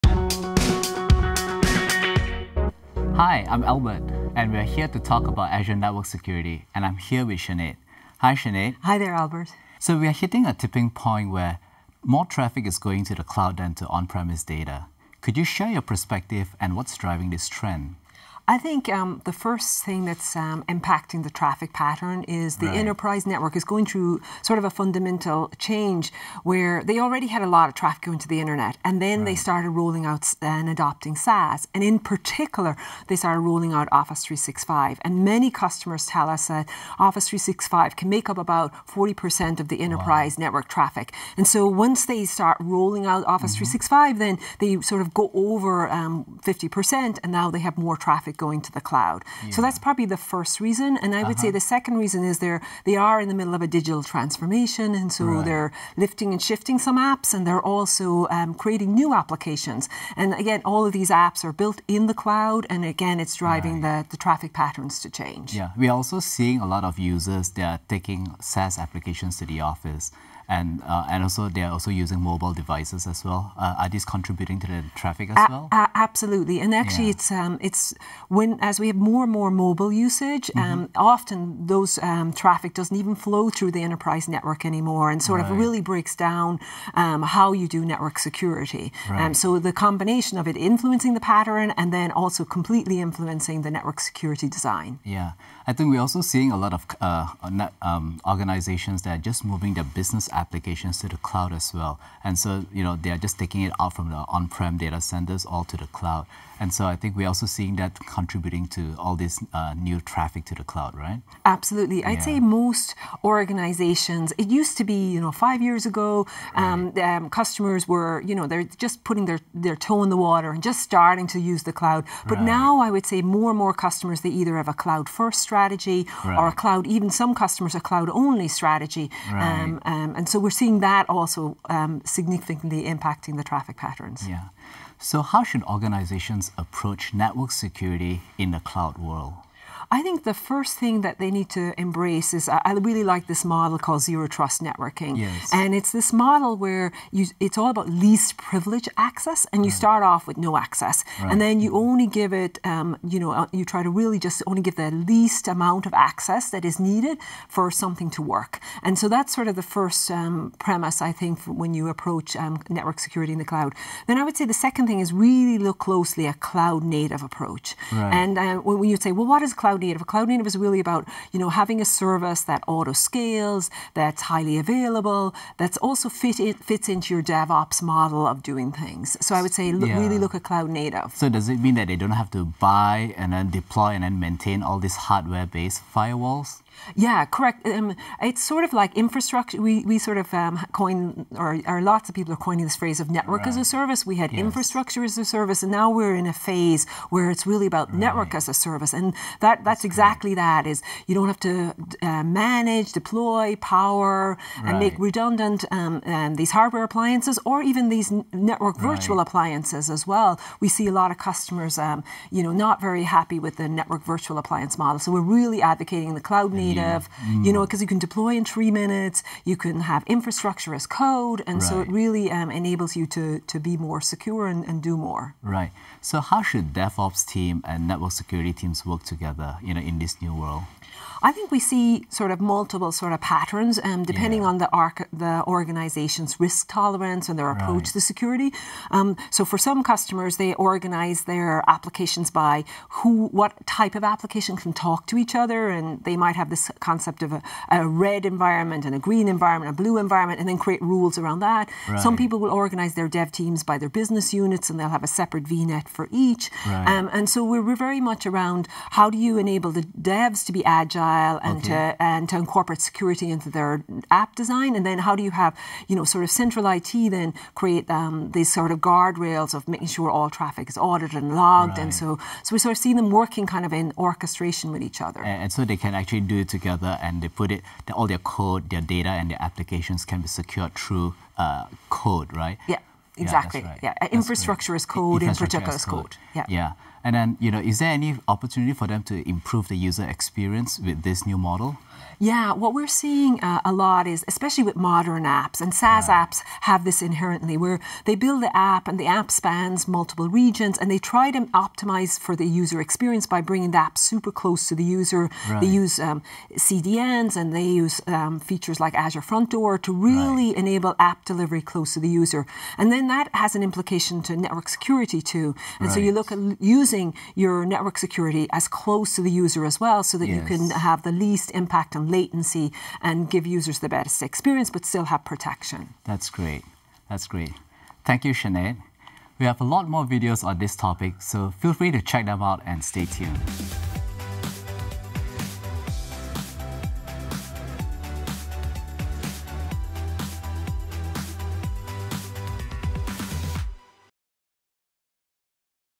What is cloud-native Azure Network Security | Azure Network Security Part 1
In this video, learn how Azure Network Security works.

We would really appreciate your feedback, please take the survey to share feedback on the session with us